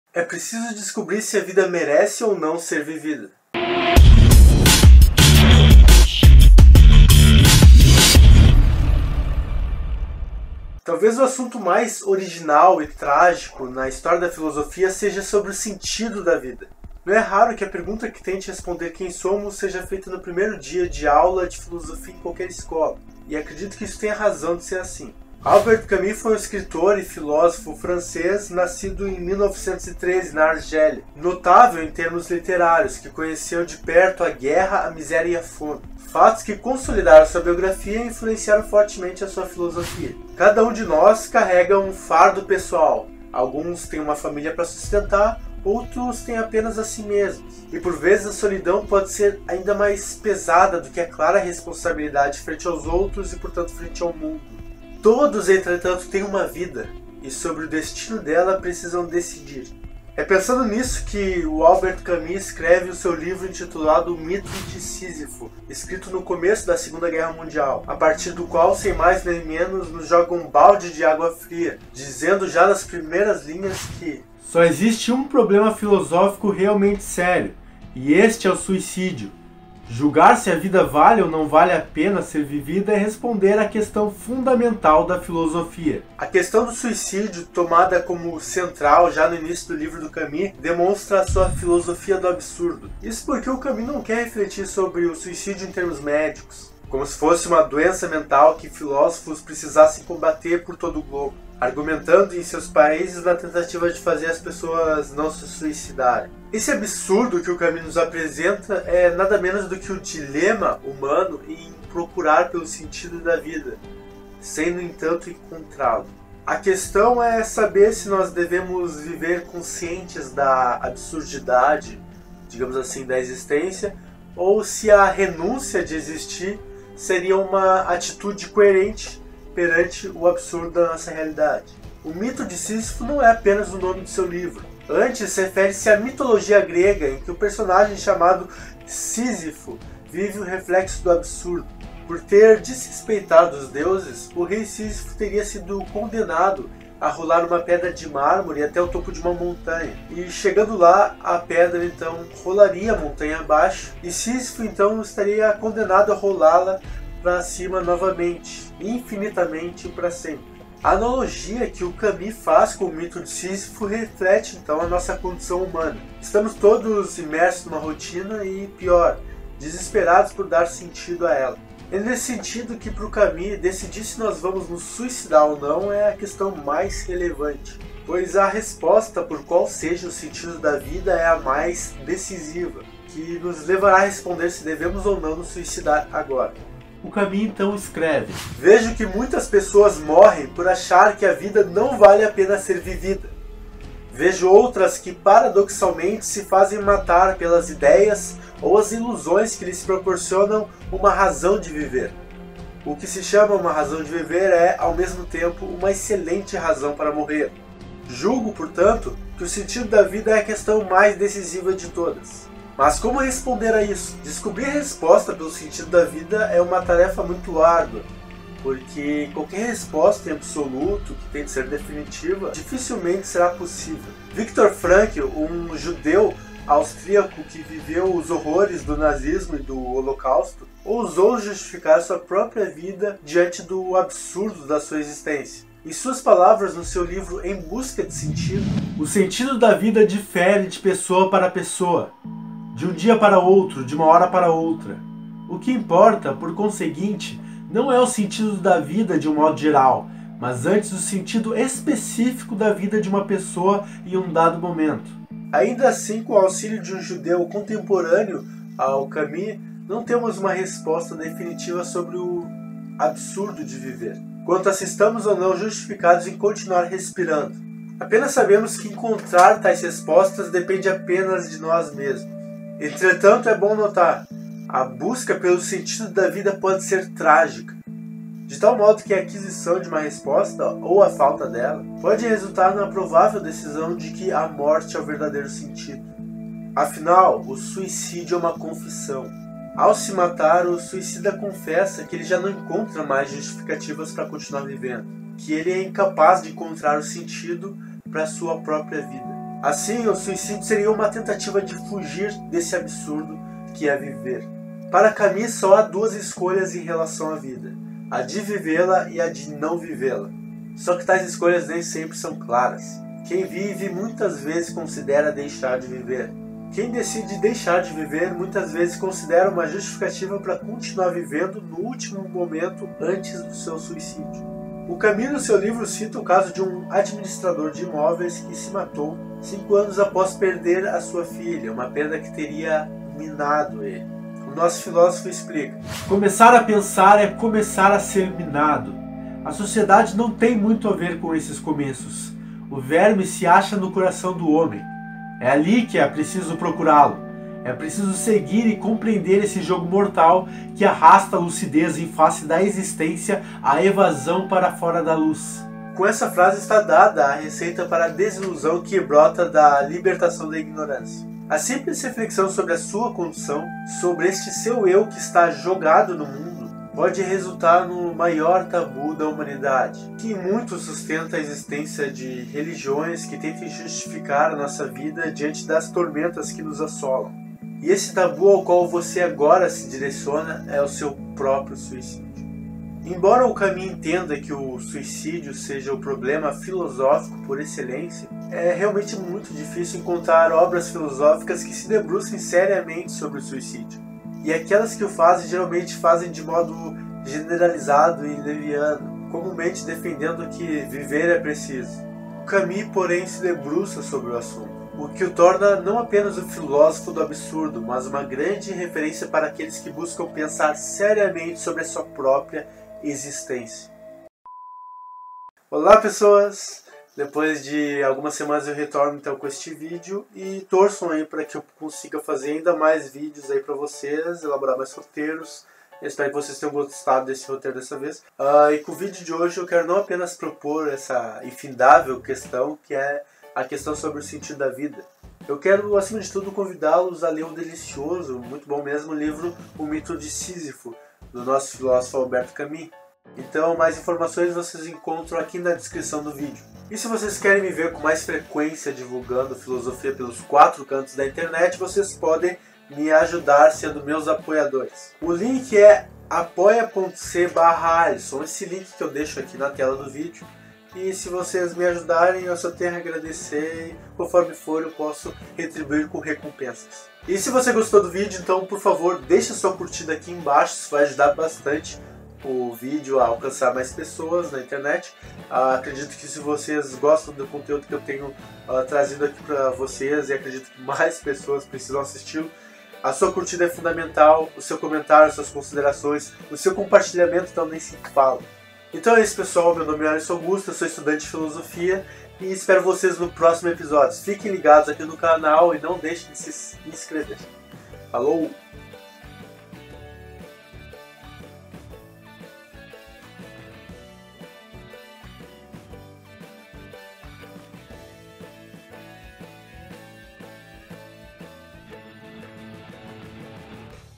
Albert Camus e a questão mais absurda de todas
Albert Camus foi um filósofo francês que refletiu pertinentemente sobre o suicídio, que ao seu ver é a principal questão filosófica. Afinal, vale a pena viver?